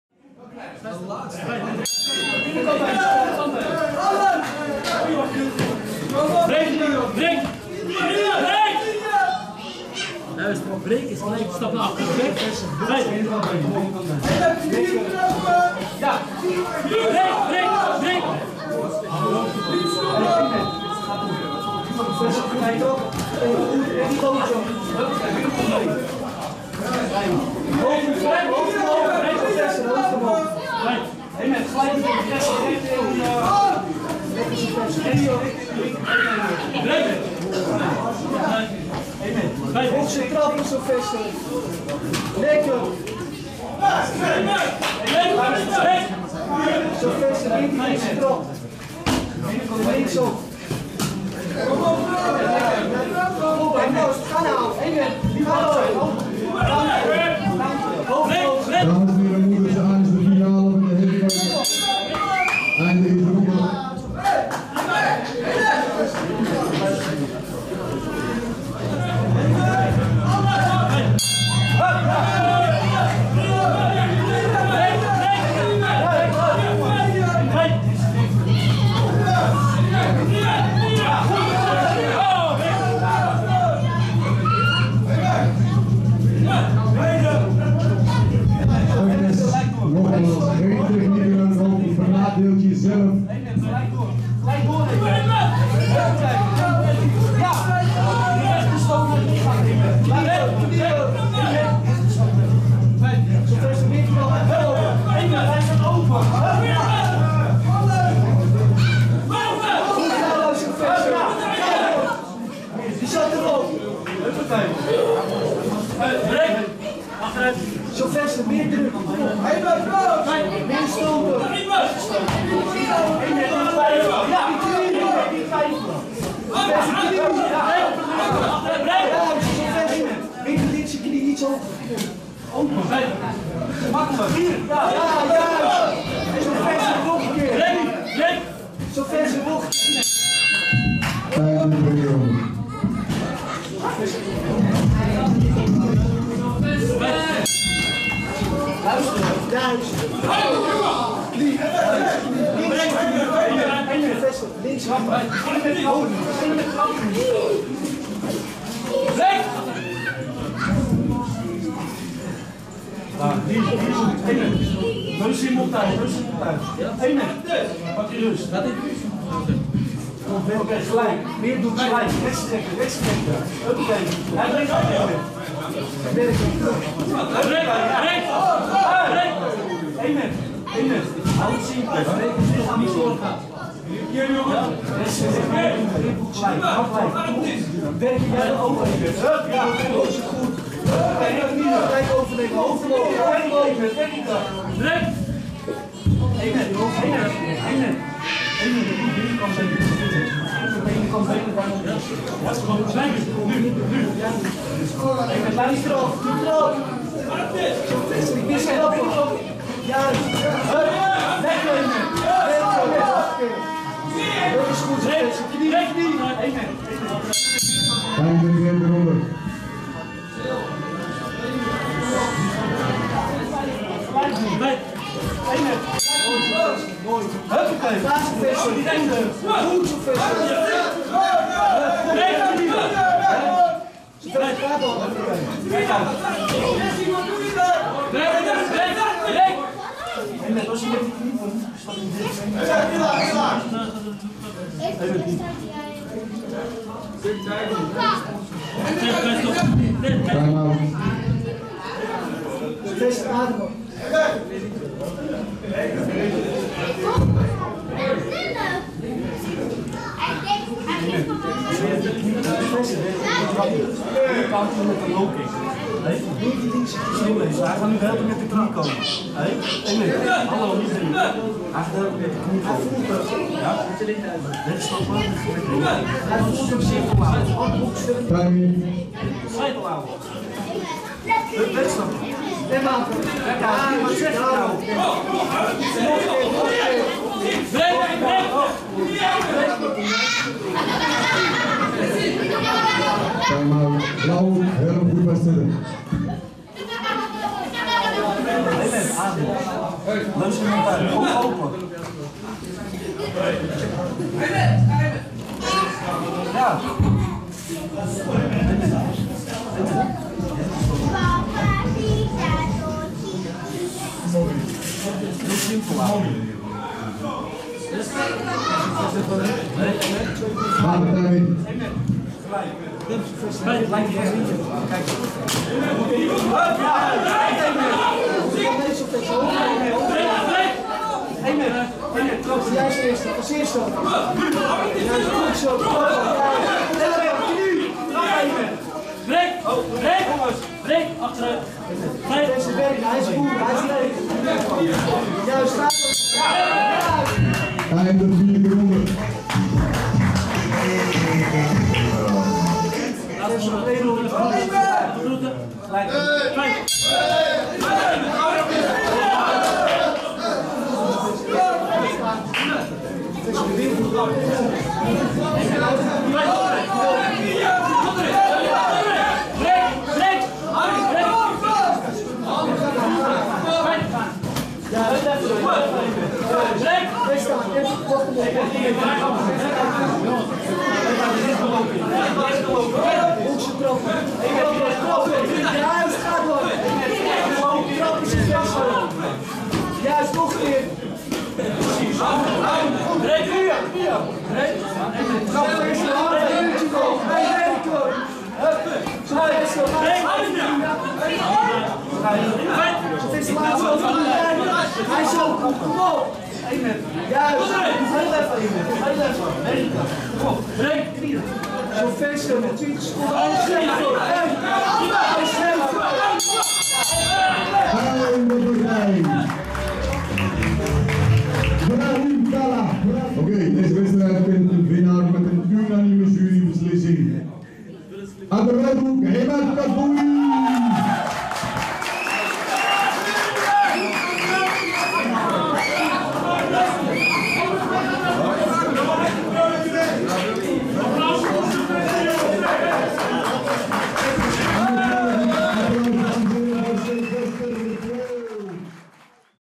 LITTLE ROLE MODELS - Imad Kaddouri vs Sylvester van der Spek
Jeugd partij 5x1 minuut.  (Finale +60 kg)
Little Role Models - Prince Of Schalkwijk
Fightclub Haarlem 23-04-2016